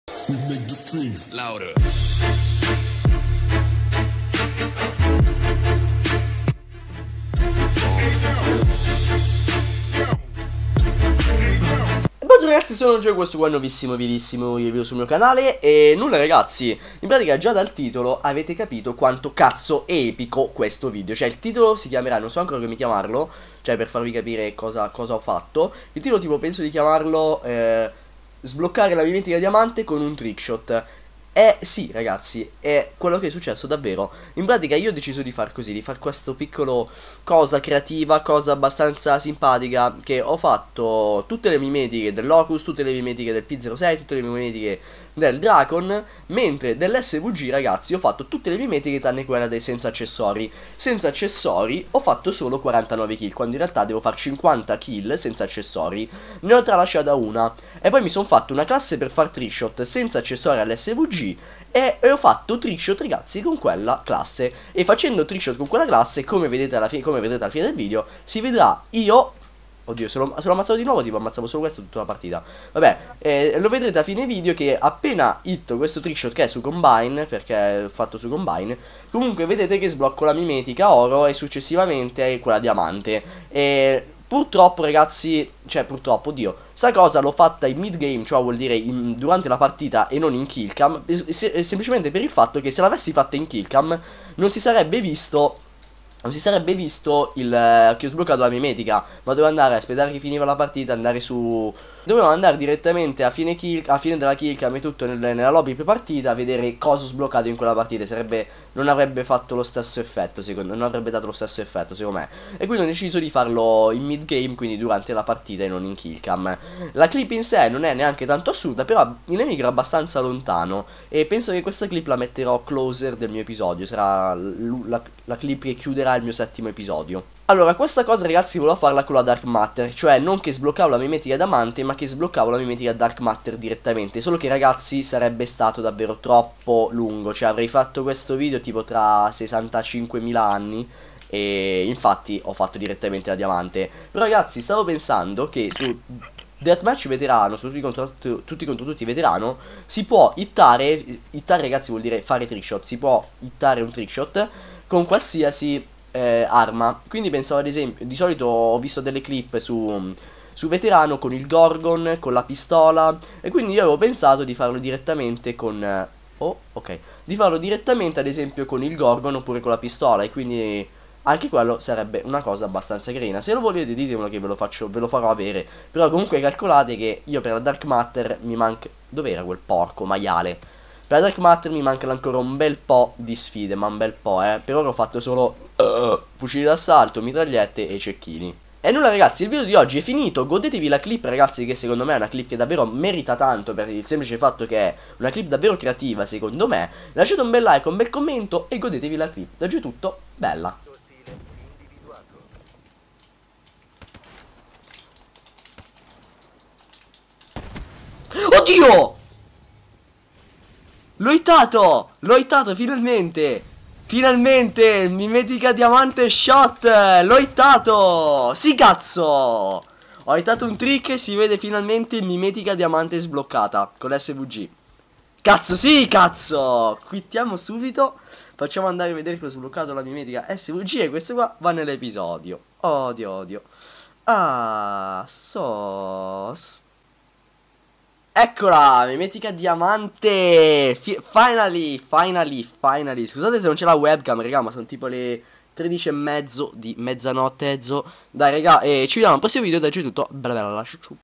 SBLOCCARE LA MIMETICA DIAMANTE CON UN TRICKSHOT!
1000 likes e vi porto un trick con il gorgon su veterano con cui sblocco la dark matter! (devo ancora farlo)

SOCIAL NETWORKS!

Psn: Gioee8

Xbox Live: Gioee

Skype: gioee8